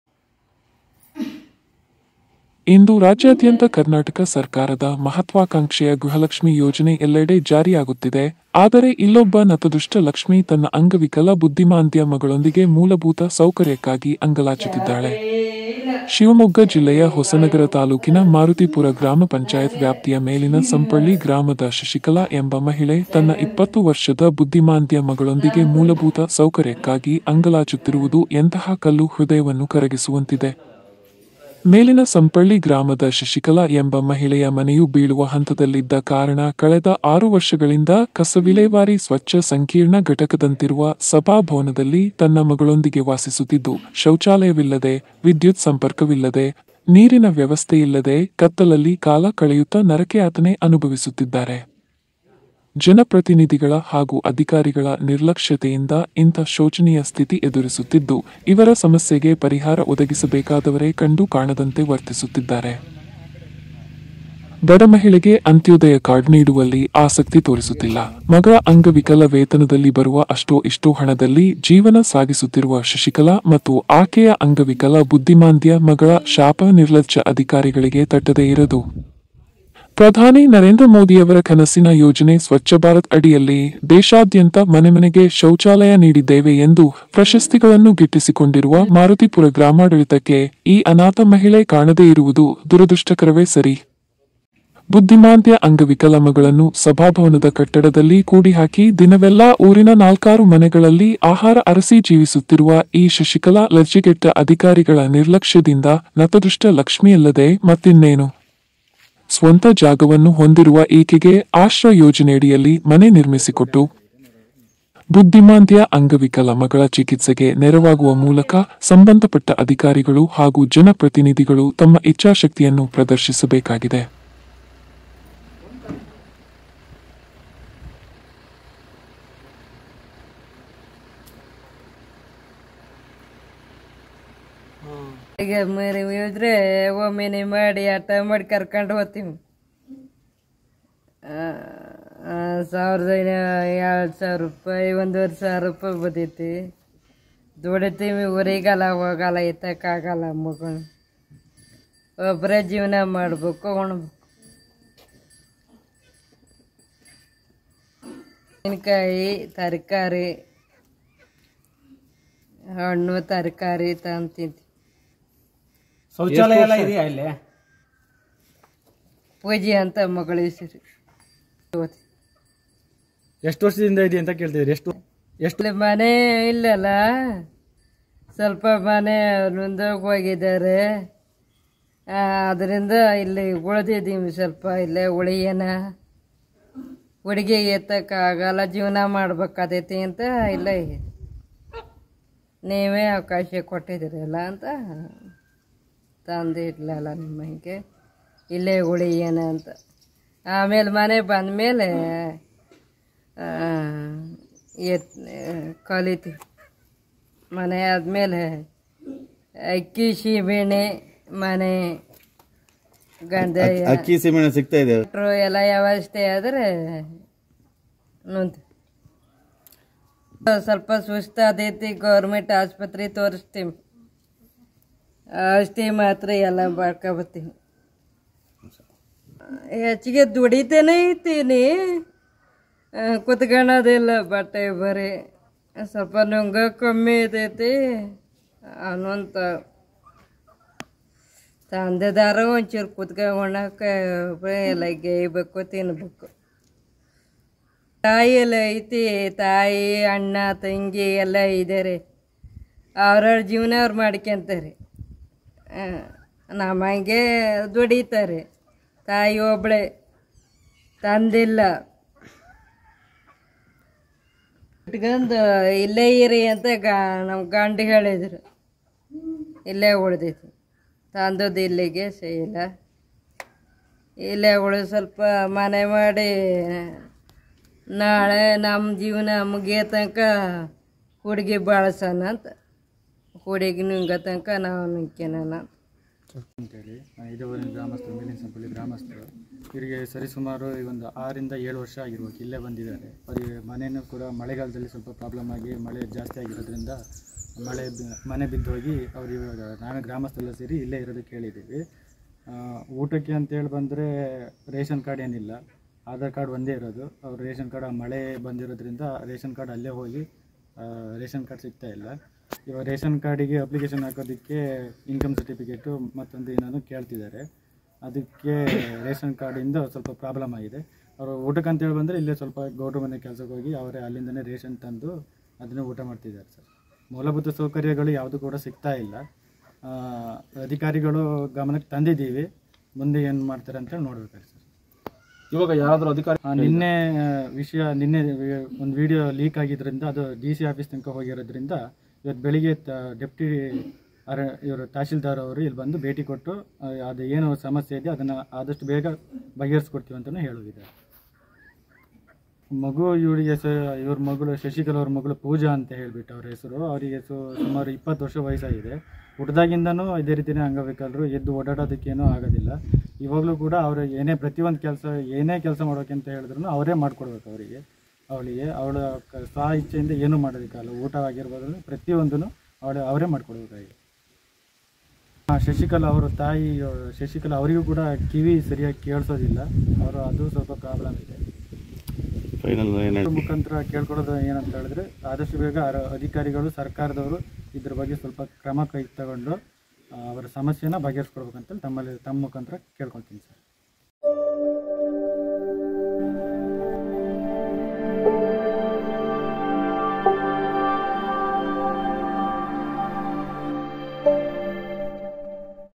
ಗೃಹಲಕ್ಷ್ಮಿಯರ ರಾಜ್ಯದಲ್ಲೊಬ್ಬಳು ನತದೃಷ್ಟ ಲಕ್ಷ್ಮಿ|ಬುದ್ದಿಮಾಂದ್ಯ ಅಂಗವಿಕಲ ಮಗಳೊಂದಿಗಿರುವ ಬಡ ಮಹಿಳೆಯ ನರಕಯಾತನೆ
ಬುದ್ದಿಮಾಂದ್ಯ ಅಂಗವಿಕಲ ಮಗಳೊಂದಿಗೆ ವಾಸಿಸುತ್ತಿರುವ ಬಡ ಮಹಿಳೆಯ ಬದುಕಿಗೆ ಸಿಕ್ಕಿತೇ ಆಸರೆ?| 6ವರ್ಷಗಳಿಂದ ಸ್ವ‍‍ಚ್ಚ ಸಂಕೀರ್ಣ ಕಟ್ಟಡದಲ್ಲಿ ವಾಸ| ಅಂತ್ಯೋದಯ ಕಾರ್ಡ್ ಇಲ್ಲ | ಮಗಳ ಚಿಕಿತ್ಸೆಗೆ ಸ್ಪಂದಿಸುವವರಿಲ್ಲ

ಇಂದು ರಾಜ್ಯಾದ್ಯಂತ ಕರ್ನಾಟಕ ಸರ್ಕಾರದ ಮಹತ್ವಾಕಾಂಕ್ಷೆಯ  ಗೃಹಲಕ್ಷ್ಮಿ ಯೋಜನೆ ಎಲ್ಲೆಡೆ ಜಾರಿಯಾಗುತ್ತಿದೆ ಆದರೆ ಇಲ್ಲೊಬ್ಬ ನತದೃಷ್ಟ ಲಕ್ಷ್ಮಿ ತನ್ನ ಅಂಗವಿಕಲ ಬುದ್ದಿಮಾಂದ್ಯ ಮಗಳೊಂದಿಗೆ ಮೂಲಭೂತ ಸೌಕರ್ಯಕ್ಕಾಗಿ ಅಂಗಲಾಚುತಿದ್ದಾಳೆ.

ಶಿವಮೊಗ್ಗ ಜಿಲ್ಲೆಯ ಹೊಸನಗರ ತಾಲೂಕಿನ ಮಾರುತಿಪುರ ಗ್ರಾಪಂ ವ್ಯಾಪ್ತಿಯ ಮೇಲಿನ ಸಂಪಳ್ಳಿ‌ ಗ್ರಾಮದ ಶಶಿಕಲಾ ಎಂಬ ಮಹಿಳೆ ತನ್ನ 20 ವರ್ಷದ ಬುದ್ದಿಮಾಂದ್ಯ ಮಗಳಾದ ಪೂಜಾಳೊಂದಿಗೆ. ಮೂಲಭೂತ ಸೌಕರ್ಯಕ್ಕಾಗಿ ಅಂಗಲಾಚುತ್ತಿರುವುದು ಎಂತಹ ಕಲ್ಲು ಹೃದಯವನ್ನು ಕರಗಿಸುವಂತಿದೆ.

ಮೇಲಿನ ಸಂಪಳ್ಳಿ ಗ್ರಾಮದ ಶಶಿಕಲಾ ಎಂಬ ಮಹಿಳೆಯ ಮನೆಯು ಬೀಳುವ ಹಂತದಲ್ಲಿದ್ದ ಕಾರಣ ಕಳೆದ ಆರು ವರ್ಷಗಳಿಂದ ಕಸ ವಿಲೇವಾರಿ ಸ್ವಚ್ಚ ಸಂಕೀರ್ಣ ಘಟಕದಂತಿರುವ ಸಭಾ ಭವನದಲ್ಲಿ ತನ್ನ ಮಗಳೊಂದಿಗೆ ವಾಸಿಸುತಿದ್ದು ಶೌಚಾಲಯವಿಲ್ಲದೇ,ವಿದ್ಯುತ್ ಸಂಪರ್ಕವಿಲ್ಲದೇ , ನೀರಿನ ವ್ಯವಸ್ಥೆಯಿಲ್ಲದೇ ಕತ್ತಲಲ್ಲಿ ಕಾಲ ಕಳೆಯುತ್ತ ನರಕಯಾತನೆ ಅನುಭವಿಸುತ್ತಿದ್ದಾರೆ.

ಜನಪ್ರತಿನಿಧಿಗಳ ಹಾಗೂ ಅಧಿಕಾರಿಗಳ ನಿರ್ಲಕ್ಷ್ಯತೆಯಿಂದ ಇಂಥ ಶೋಚನೀಯ ಸ್ಥಿತಿ ಎದುರಿಸುತ್ತಿದ್ದು, ಇವರ ಸಮಸ್ಯೆಗೆ ಪರಿಹಾರ ಒದಗಿಸಬೇಕಾದವರೇ ಕಂಡೂ ಕಾಣದಂತೆ ವರ್ತಿಸುತ್ತಿದ್ದಾರೆ.

ಬಲಾಢ್ಯರಿಗೆಲ್ಲಾ ಬಿಪಿ ಎಲ್ ಕಾರ್ಡ್ ನೀಡುವ ಅಧಿಕಾರಿಗಳು ಇಂತಹ ಬಡ ಮಹಿಳೆಗೆ ಅಂತ್ಯೋದಯ ಕಾರ್ಡ್ ನೀಡುವಲ್ಲಿ ಆಸಕ್ತಿ ತೋರಿಸುತ್ತಿಲ್ಲ.ಮಗಳ ಅಂಗವಿಕಲ ವೇತನದಲ್ಲಿ ಬರುವ ಅಷ್ಟೋ ಇಷ್ಟೋ ಹಣದಲ್ಲಿ ಜೀವನ ಸಾಗಿಸುತ್ತಿರುವ ಶಶಿಕಲಾ ಮತ್ತು ಆಕೆಯ ಅಂಗವಿಕಲ ಬುದ್ದಿಮಾಂದ್ಯ ಮಗಳ ಶಾಪ ನಿರ್ಲಜ್ಜ ಅಧಿಕಾರಿಗಳಿಗೆ ತಟ್ಟದೇ ಇರದು.

ಪ್ರಧಾನಿ ನರೇಂದ್ರ ಮೋದಿಯವರ ಕನಸಿನ ಯೋಜನೆ ಸ್ವಚ್ಚಭಾರತ್ ಅಡಿಯಲ್ಲಿ ದೇಶಾದ್ಯಂತ ಮನೆಮನೆಗೆ ಶೌಚಾಲಯ ನೀಡಿದ್ದೇವೆ ಎಂದು ಪ್ರಶಸ್ತಿಗಳನ್ನು ಗಿಟ್ಟಿಸಿಕೊಂಡಿರುವ ಮಾರುತಿಪುರ ಗ್ರಾಮಾಡಳಿತಕ್ಕೆ ಈ ಅನಾಥ ಮಹಿಳೆ ಕಾಣದೇ ಇರುವುದು ದುರದೃಷ್ಟಕರವೇ ಸರಿ. !!!

ಬುದ್ದಿಮಾಂದ್ಯ ಅಂಗವಿಕಲ ಮಗಳನ್ನು ಸಭಾಭವನದ ಕಟ್ಟಡದಲ್ಲಿ ಕೂಡಿಹಾಕಿ ದಿನವೆಲ್ಲಾ ಊರಿನ ನಾಲ್ಕಾರು ಮನೆಗಳಲ್ಲಿ ಆಹಾರ ಅರಸಿ ಜೀವಿಸುತ್ತಿರುವ ಈ ಶಶಿಕಲಾ ಲಜ್ಜೆಗೆಟ್ಟ ಅಧಿಕಾರಿಗಳ ನಿರ್ಲಕ್ಷ್ಯದಿಂದ ನತದೃಷ್ಟ ಲಕ್ಷ್ಮಿಯಲ್ಲದೇ ಮತ್ತಿನ್ನೇನು...!!??

ಸ್ವಂತ ಜಾಗವನ್ನು ಹೊಂದಿರುವ ಈಕೆಗೆ ಆಶ್ರ ಯೋಜನೆಯಡಿಯಲ್ಲಿ‌ ಮನೆ ನಿರ್ಮಿಸಿಕೊಟ್ಟು ,ಬುದ್ದಿಮಾಂದ್ಯ ಅಂಗವಿಕಲ ಮಗಳ ಚಿಕಿತ್ಸೆಗೆ ನೆರವಾಗುವ ಮೂಲಕ ಸಂಬಂಧಪಟ್ಟ ಅಧಿಕಾರಿಗಳು ಹಾಗೂ ಜನಪ್ರತಿನಿಧಿಗಳು ತಮ್ಮ ಇಚ್ಚಾಶಕ್ತಿಯನ್ನು ಪ್ರದರ್ಶಿಸಬೇಕಾಗಿದೆ.